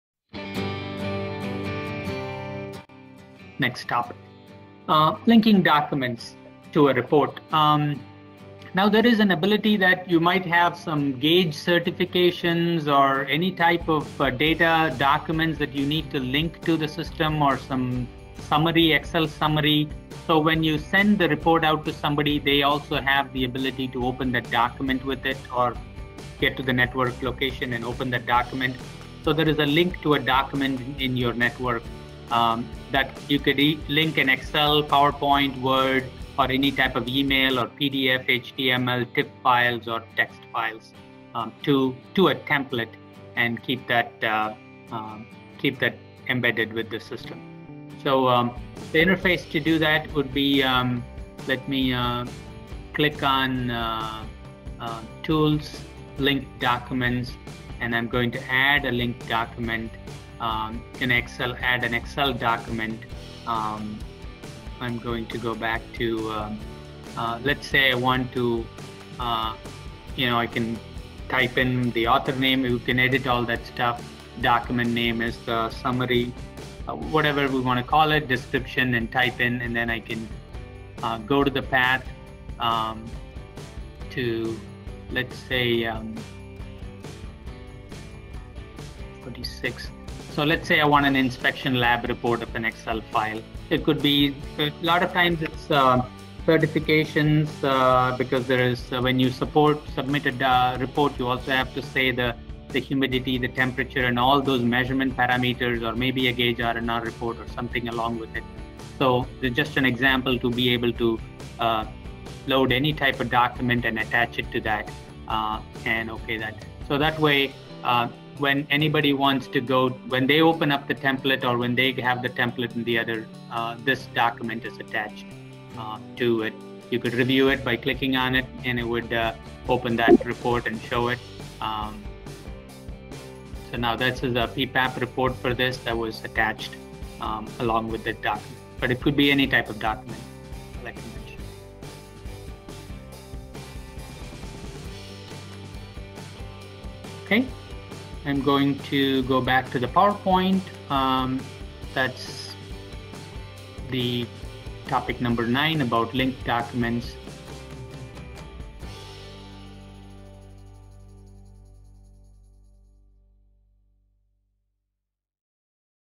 QDM SPC New Feature - 7 - Link Documents to Reports to Attach Measurement Plans and Information
This feature provides users the ability to link multiple formats of documents to the template.